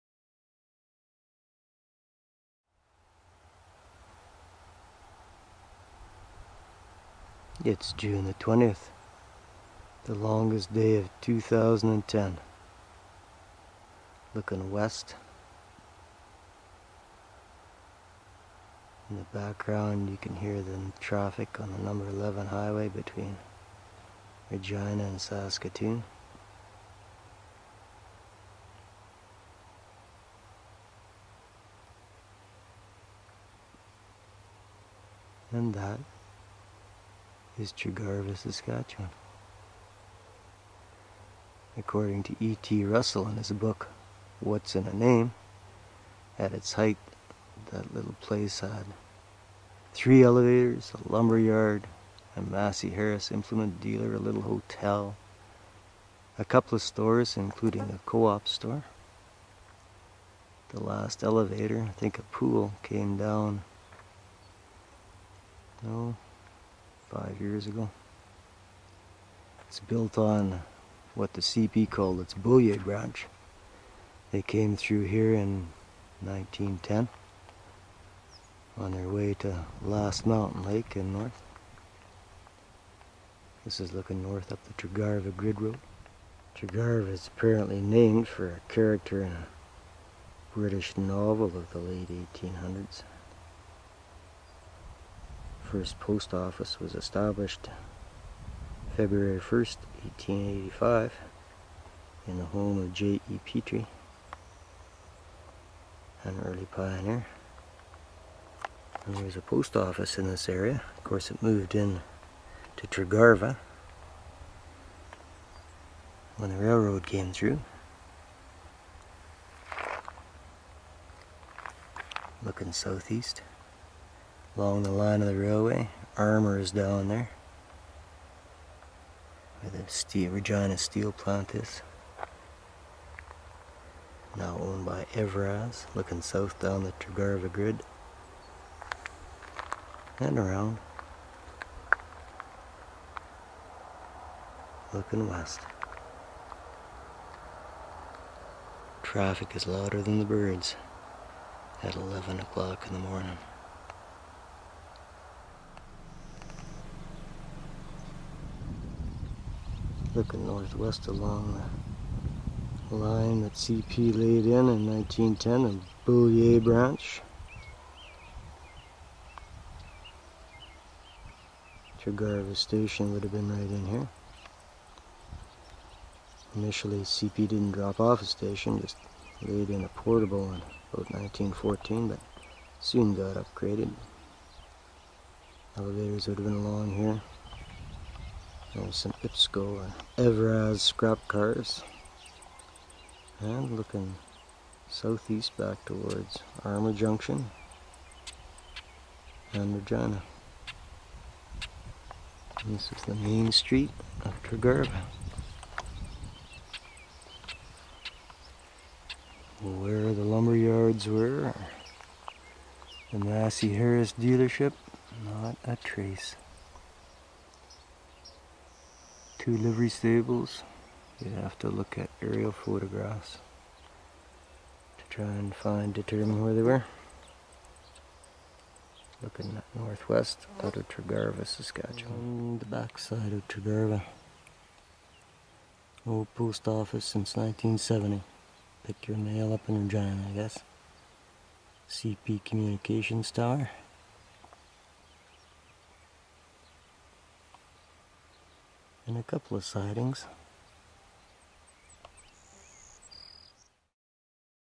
Tregarva, Saskatchewan
On a morning late in the rainiest spring on record, the sun shines on Tregarva.